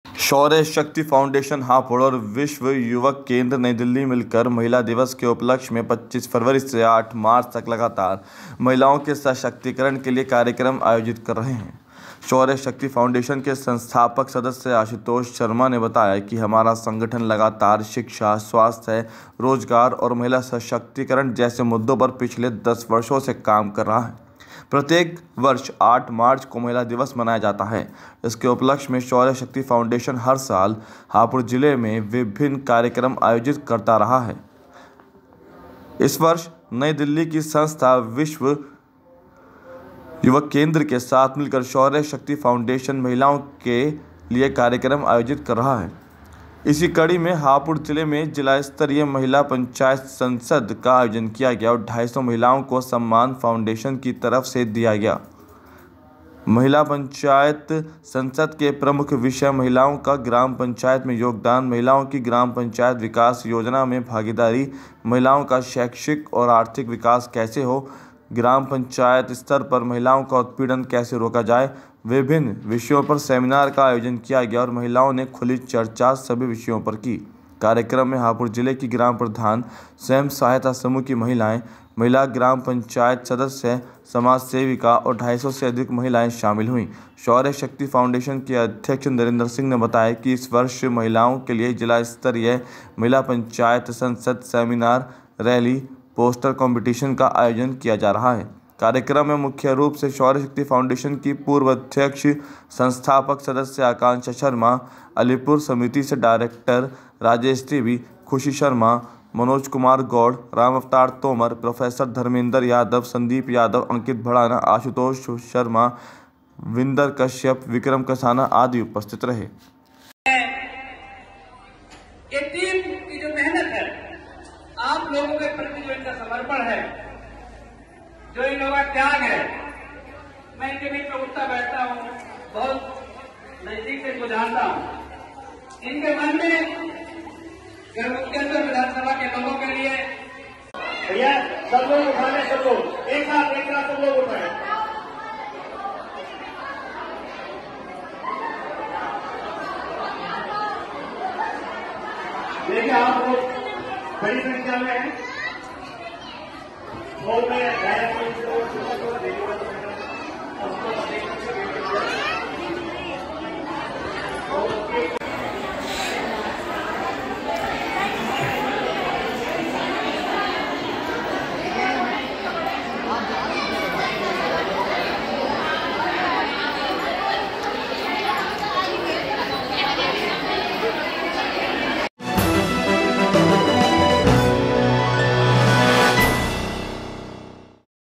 महिला दिवस के उपलक्ष्य में साप्ताहिक कार्यक्रम का आयोजन
शौर्य शक्ति फाउंडेशन हापुड़ और विश्व युवक केंद्र नई दिल्ली मिलकर महिला दिवस के उपलक्ष्य में 25 फरवरी से 8 मार्च तक लगातार महिलाओं के सशक्तिकरण के लिए कार्यक्रम आयोजित कर रहा है। शौर्य शक्ति फाउंडेशन के संस्थापक सदस्य आशुतोष शर्मा ने बताया हमारा संगठन लगातार शिक्षा, स्वास्थ्य, रोजगार और महिला सशक्तिकरण जैसे मुद्दों पर पिछले 10 वर्षों से काम कर रहा है। प्रत्येक वर्ष 8 मार्च को महिला दिवस मनाया जाता है। इसके उपलक्ष्य में शौर्य शक्ति फाउंडेशन हर साल हापुड़ जिले में विभिन्न कार्यक्रम आयोजित करता है। इस वर्ष नई दिल्ली की संस्था विश्व युवक केंद्र के साथ मिलकर शौर्य शक्ति फाउंडेशन महिलाओं के कार्यक्रम आयोजित कर रहा है।
इसी कड़ी में हापुड़ जिले में जिला स्तरीय महिला पंचायत संसद का आयोजन किया गया और 250 महिलाओं को सम्मान फाउंडेशन की तरफ से दिया गया महिला पंचायत संसद के प्रमुख विषय महिलाओं का ग्राम पंचायत में योगदान ,महिलाओं की ग्राम पंचायत विकास योजना में भागीदारी, महिलाओं का शैक्षिक एवं आर्थिक विकास कैसे हो ,ग्राम पंचायत स्तर पर महिलाओं का उत्पीड़न कैसे रोका जाए। विभिन्न विषयों पर सेमिनार का आयोजन किया गया और महिलाओं ने खुली चर्चा सभी विषयों पर की।
कार्यक्रम में हापुड़ जिले की ग्राम प्रधान, स्वयं सहायता समूह की महिलाएं, महिला ग्राम पंचायत सदस्य, समाजसेविका और 250 से अधिक महिलाएं शामिल हुई।
शौर्य फाउंडेशन के अध्यक्ष नरेंद्र सिंह ने बताया इस वर्ष हम महिलाओं के लिए जिला स्तरीय महिला पंचायत संसद, सेमिनार, रैली, एवं पोस्टर कंपटीशन का आयोजन करने जा रहे हैं।
कार्यक्रम में मुख्य रूप से शौर्य शक्ति फाउंडेशन की पूर्व अध्यक्ष एवं संस्थापक सदस्य आकांक्षा शर्मा, अलीपुर समिति से डायरेक्टर राजेश देवी, खुशी शर्मा, मनोज कुमार गौड़, राम अवतार तोमर, प्रोफेसर धर्मेंद्र, यादव, संदीप यादव, अंकित भड़ाना, आशुतोष शर्मा, विंदर कश्यप विक्रम कसाना आदि लोग मौजूद रहे।